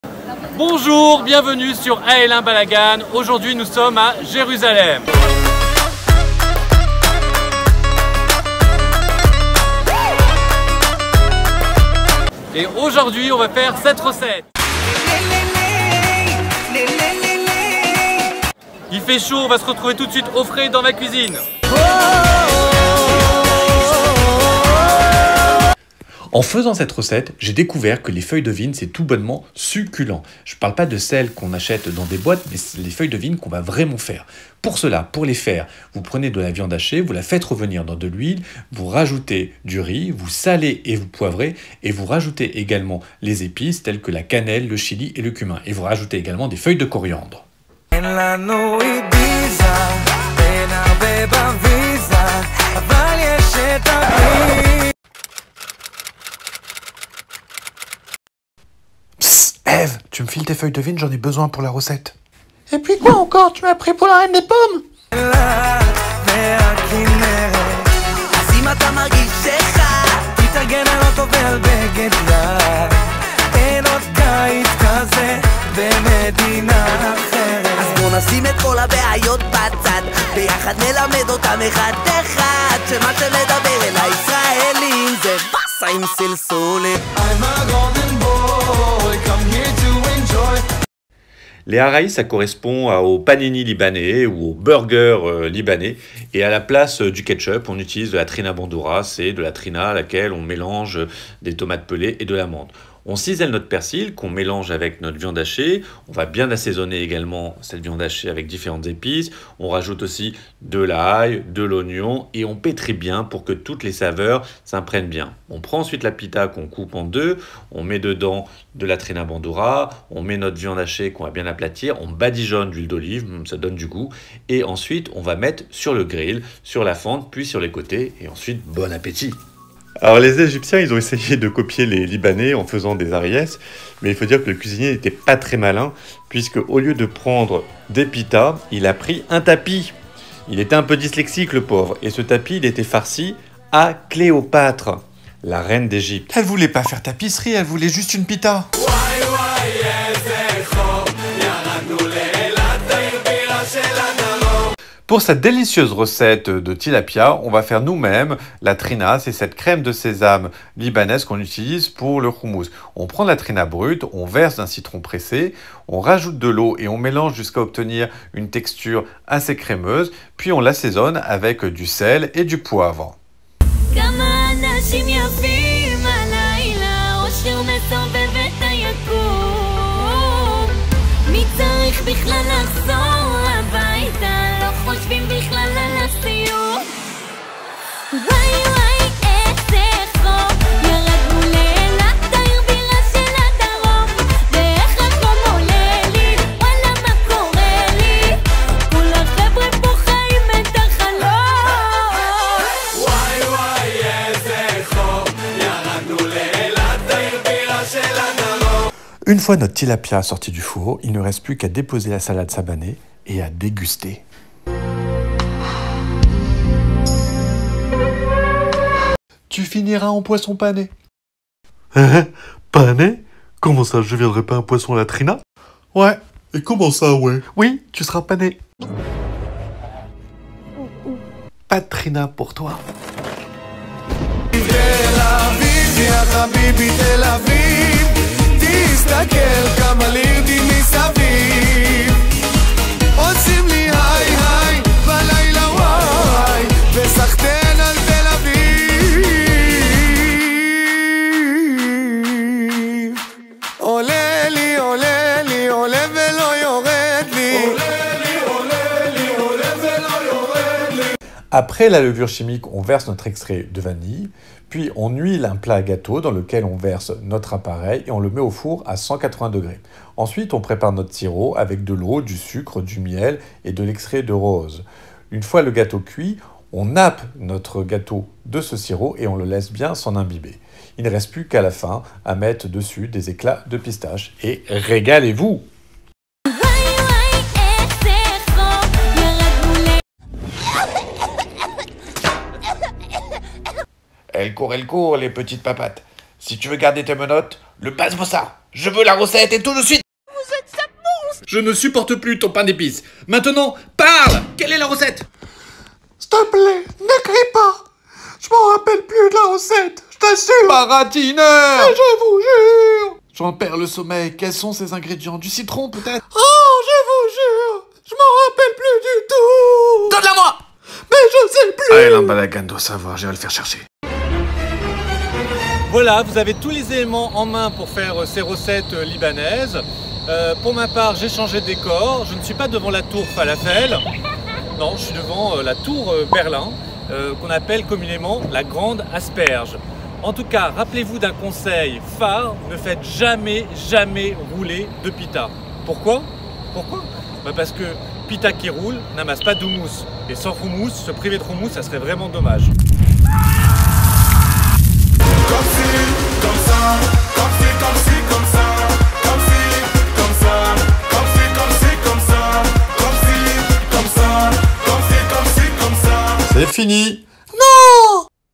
Les fabuleux mets du Liban
Au menu aujourd’hui, je propose à votre estomac de faire la danse 💃🏻 du ventre avec de «fabuleux mets🍽 du liban״

Au menu, vous apprendrez à faire des arayes 🥙, des feuilles de vigne 🌱, du tilapia 🐠 à la tehina et pour le dessert🍰 un basboussa.

Le tout accompagné d’un petit digestif humouristique 😃

Alors succombez et cliquez sur le lien ci dessous pour visionner la recette filmée

Régalez vous 😋!